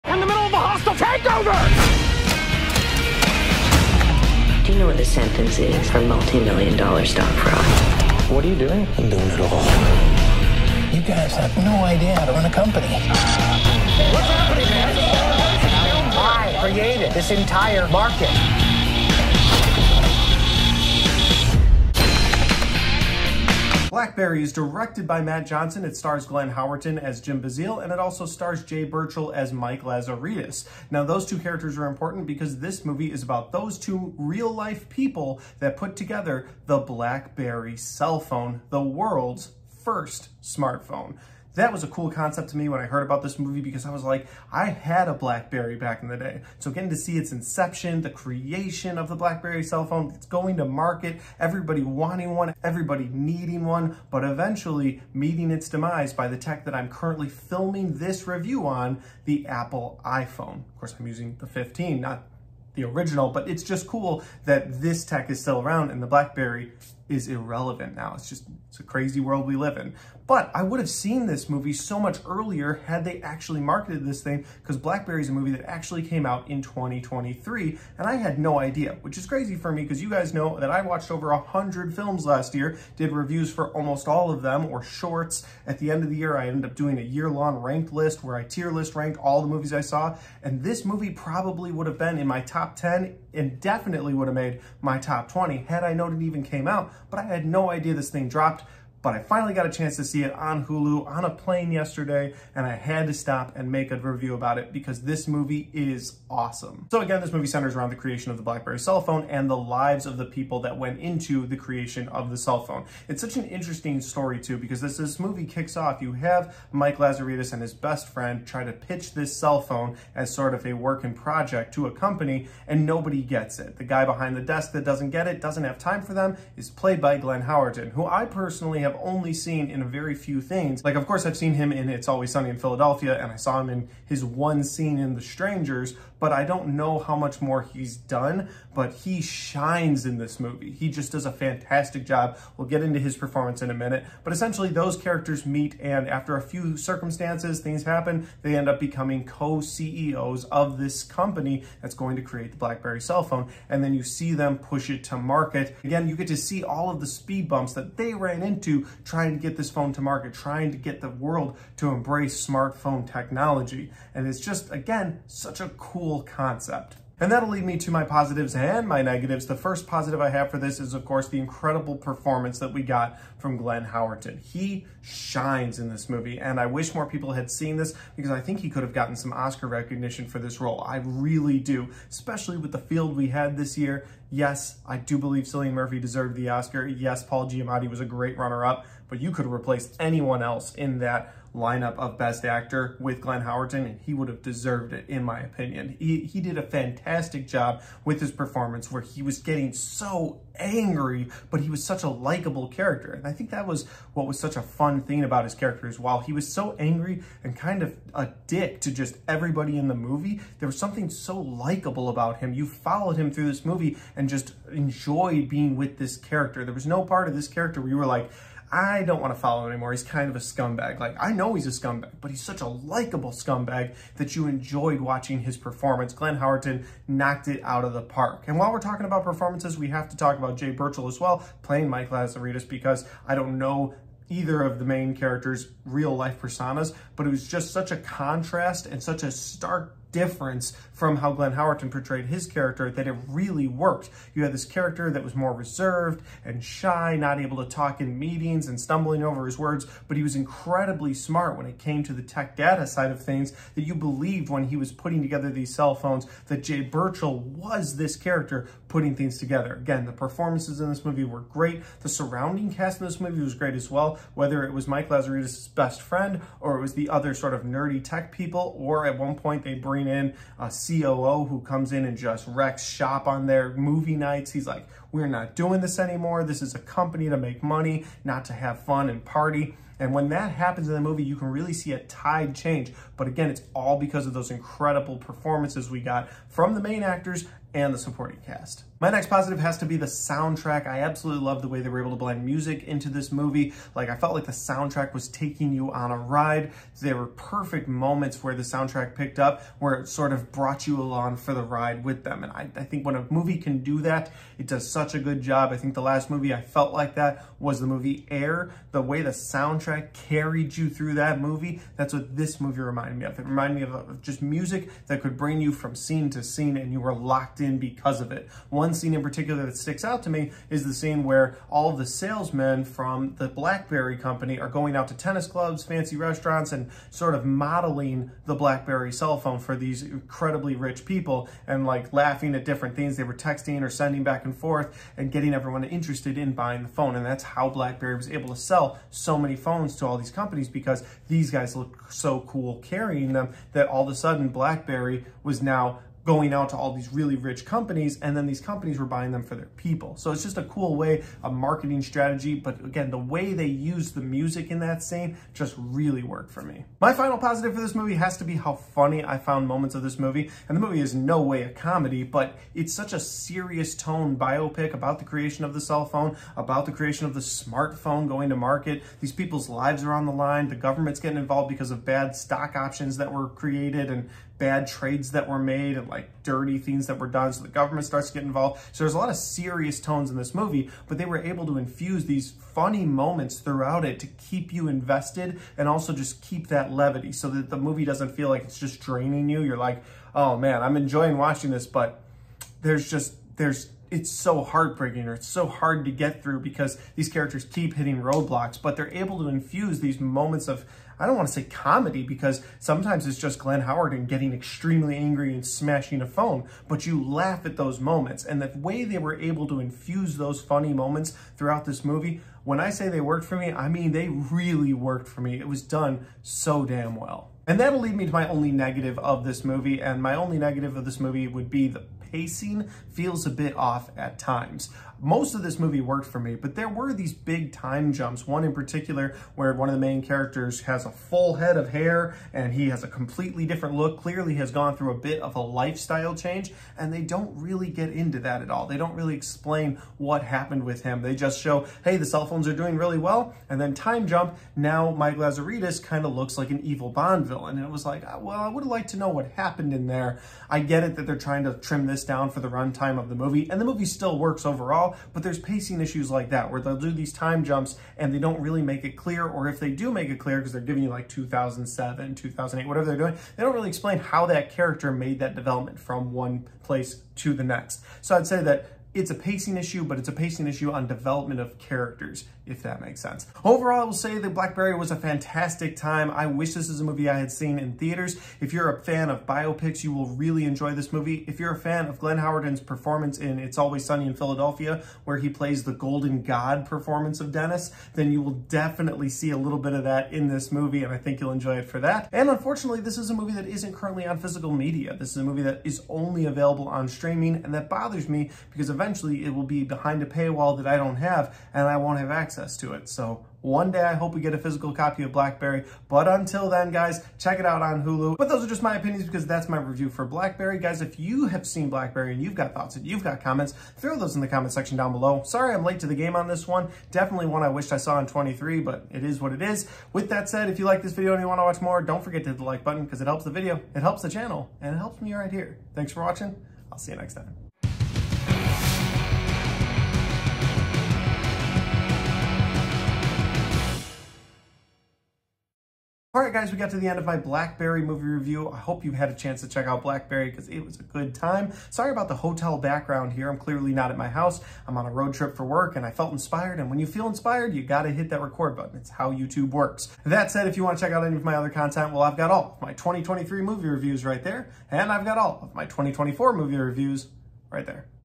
BLACKBERRY: MOVIE REVIEW
Here are my thoughts on the 2023 film Blackberry!